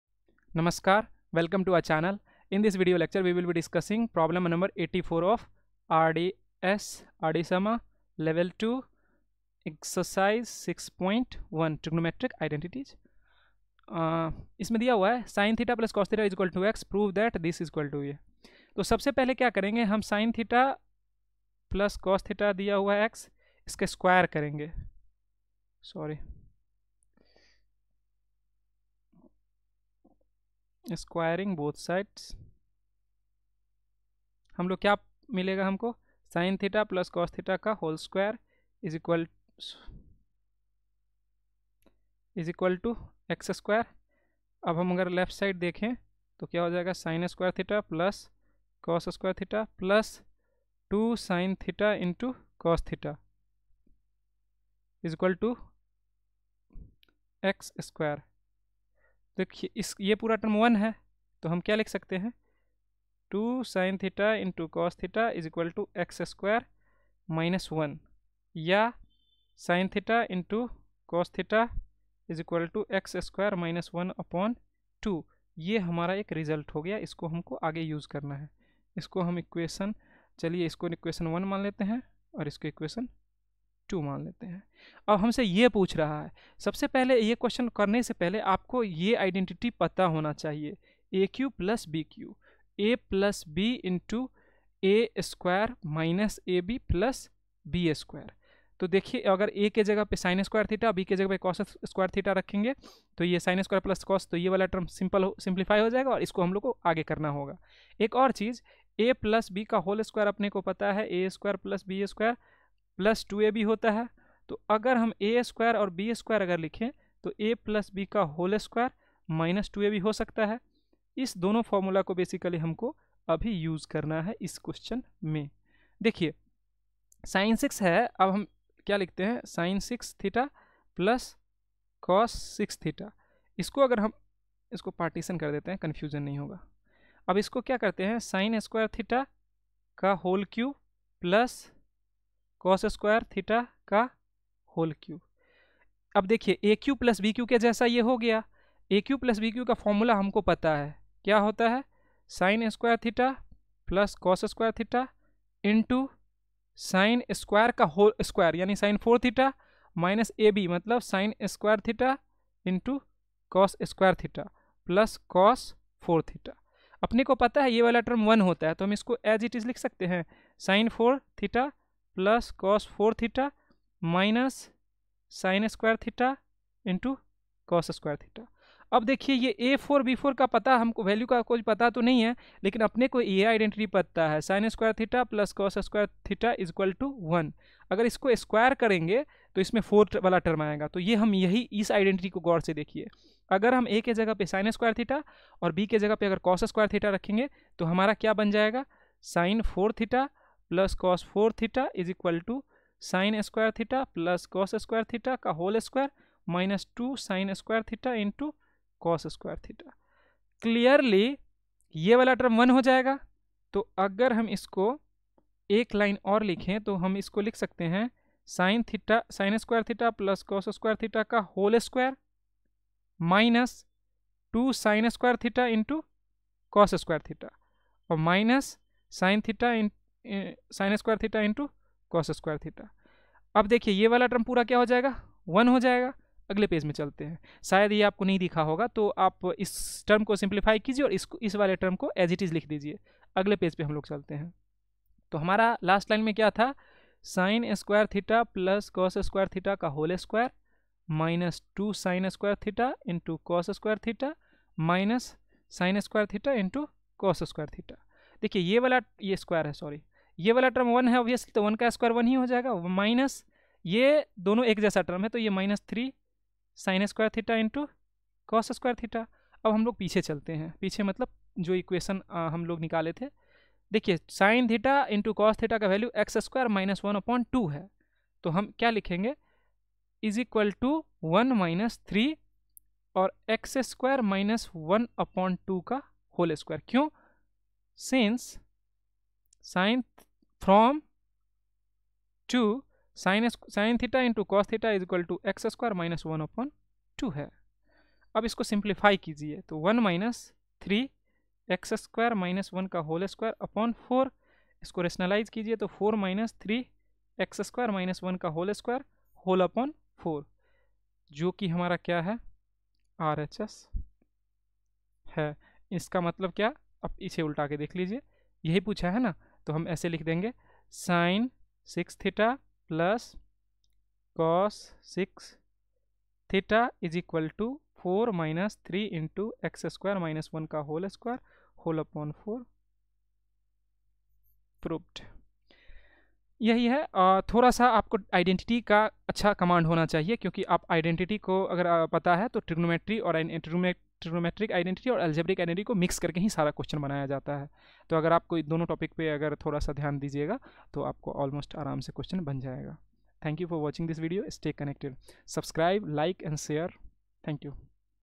Q 84, R D Sharma( Ex-6.1, Level- 2) Trigonometric Identities
Here Q 84 of Ex- 6.1, R D Sharma(Trigonometric Identities) has been discussed